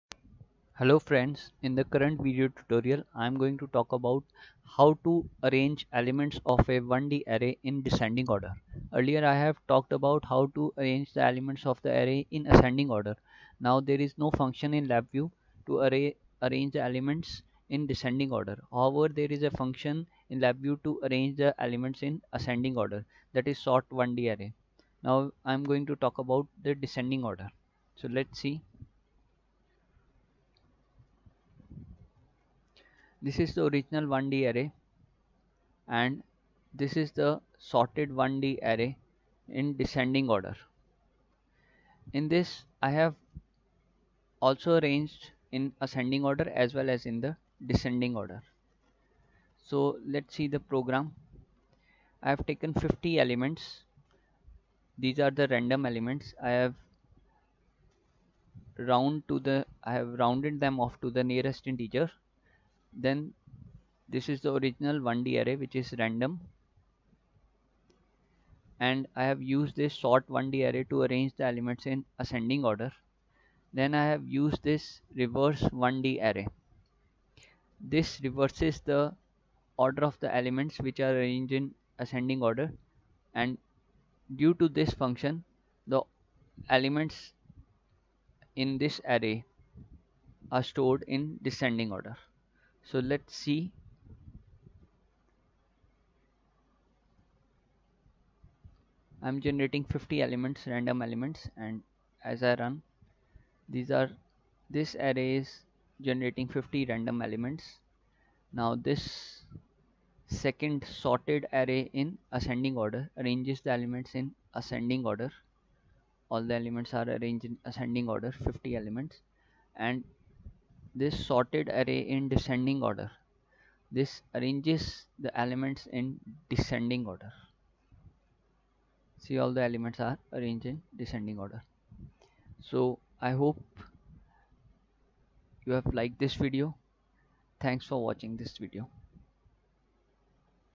How to arrange elements of a 1-D Array in descending order in LabVIEW
This video tutorial describes about how to arrange elements of a 1-D Array in descending order in LabVIEW. This video tutorial is also helpful for CLAD exam Preparation.